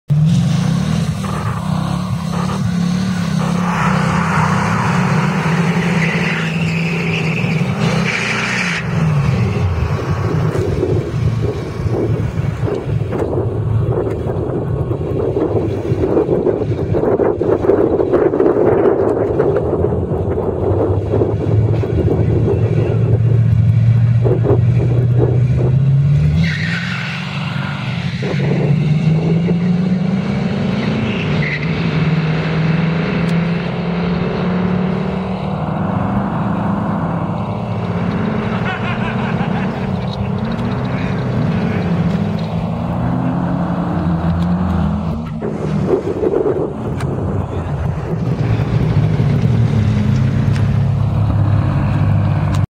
Mustang Gt-351 Vs Mustang Fox 84 (Track Day GDL)
Mustang Gt-351 Holley 4 gargantas & headers Vs Mustang Fox holley 4 gargantas & Headers corriendo en el track day en el autodromo Guadalajara